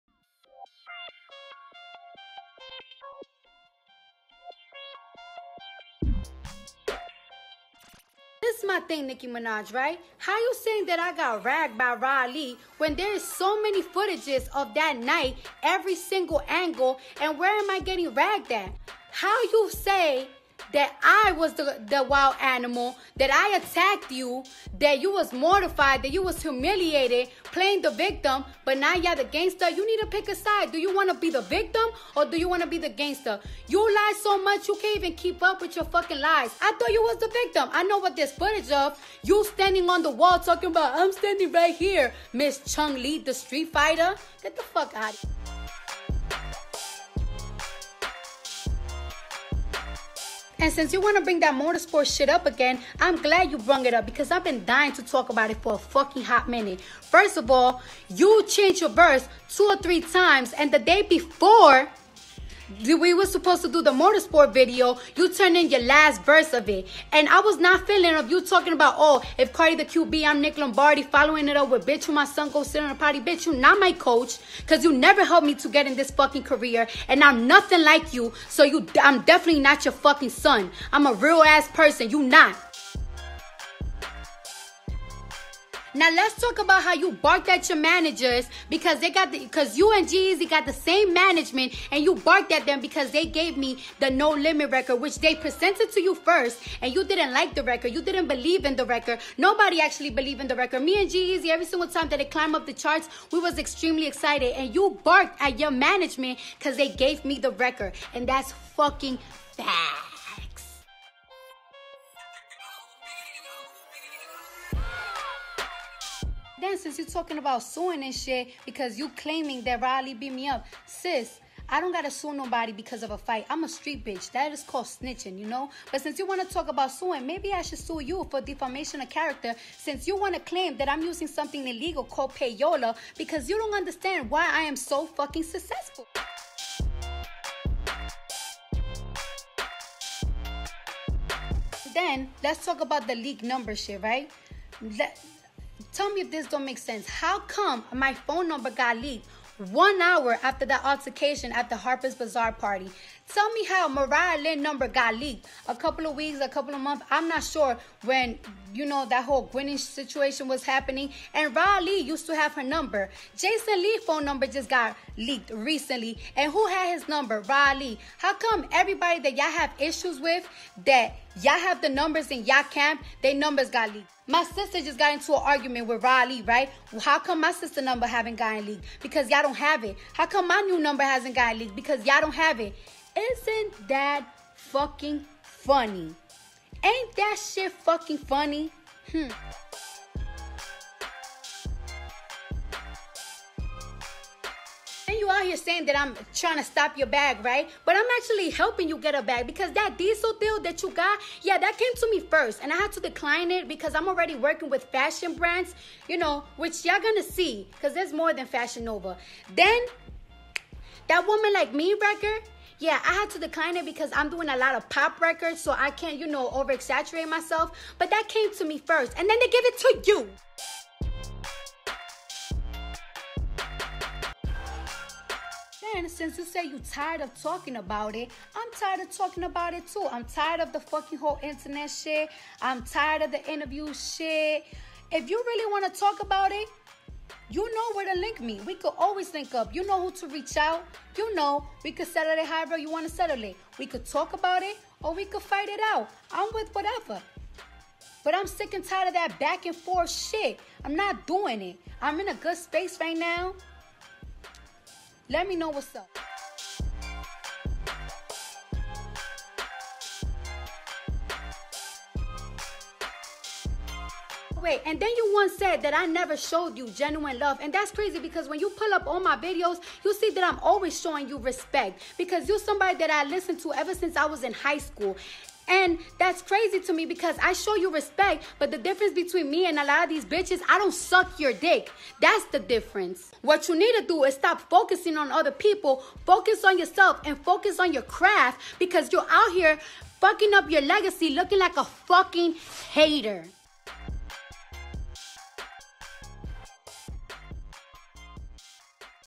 Cardi B Goes Off on Nicki Minaj
CardiB fires back at Nicki Minaj after Nicki said there is footage of Cardi being beaten up by Rah Ali.